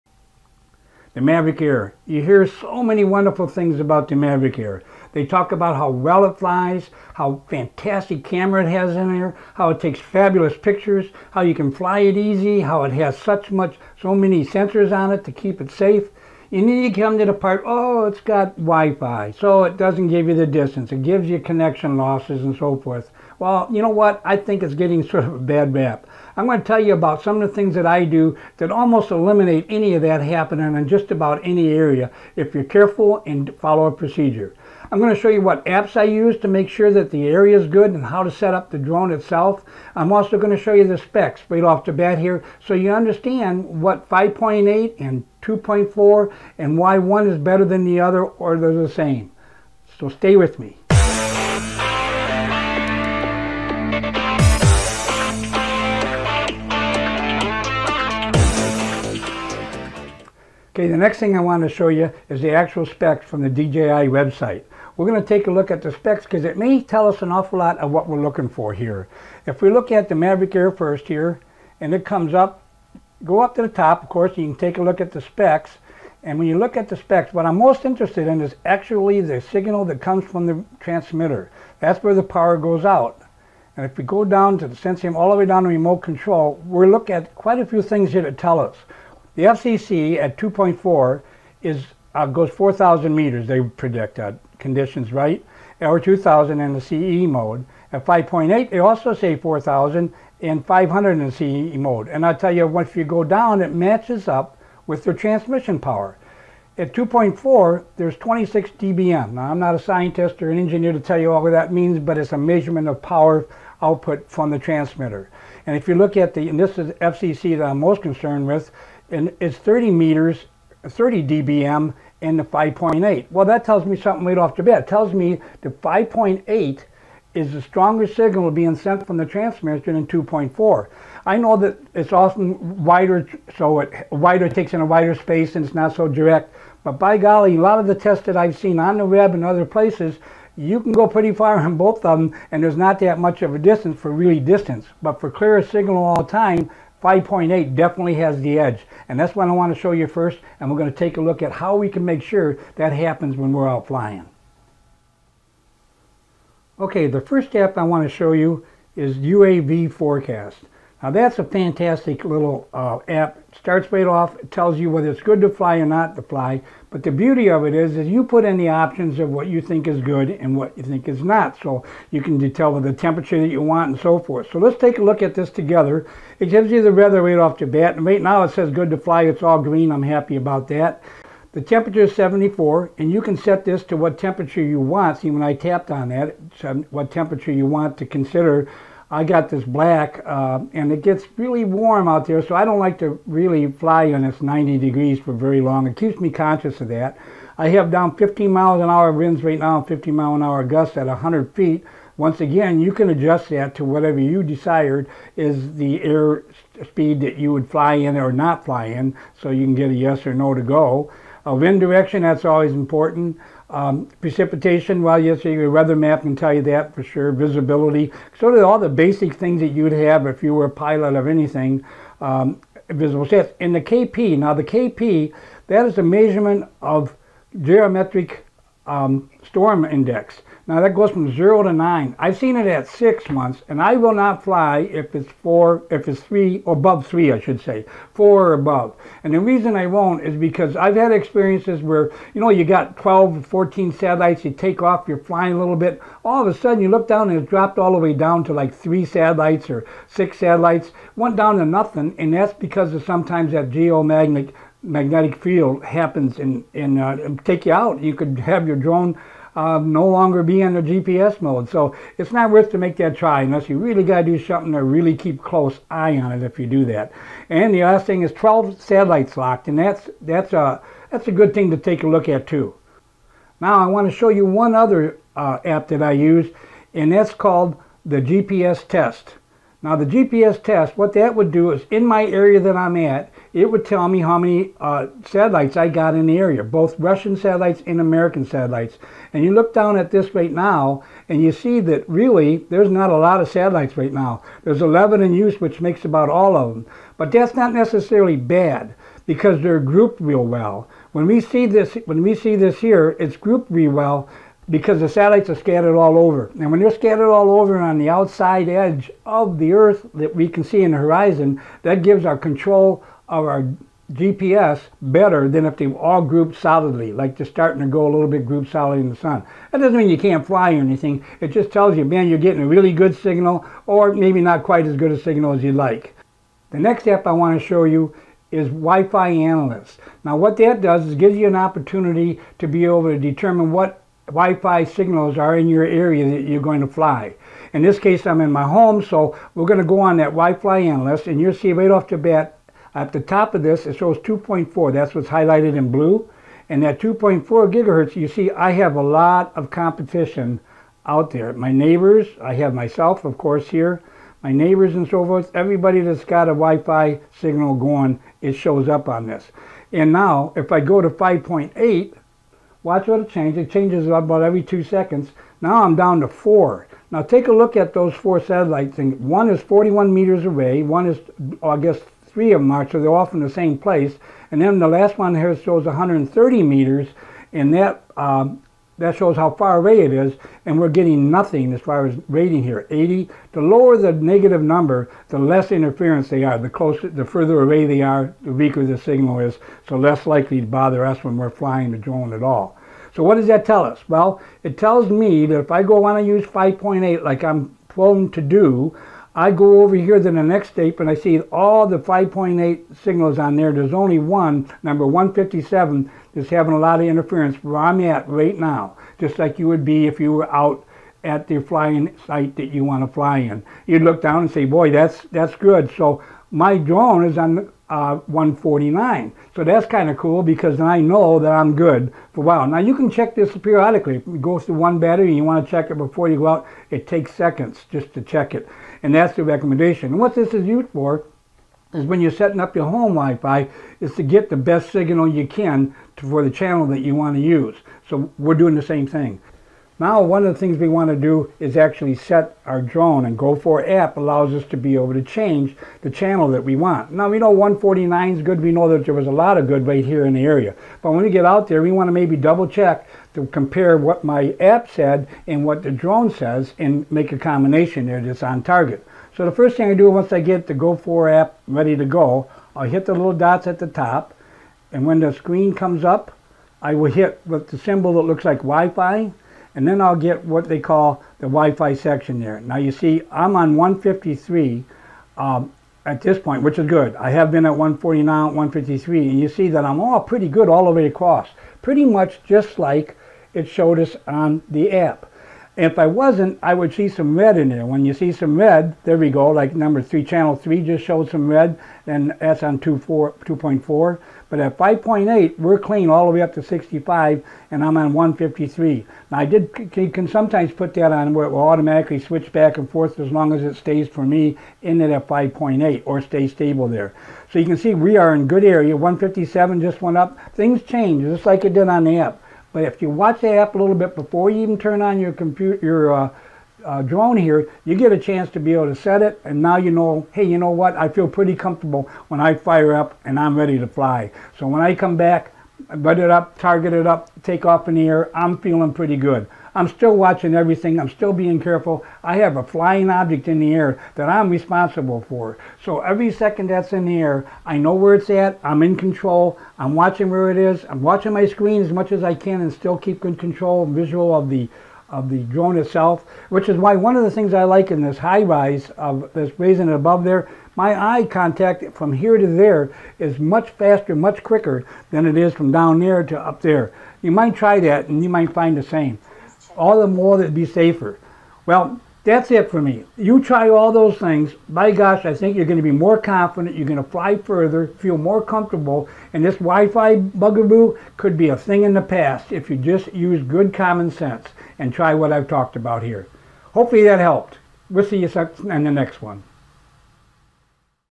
Mavic Air ... Stop losing connection
In this video I go through a series of apps to isolate and avoid wifi interference in the Mavic Air. Get safer and more secure flights.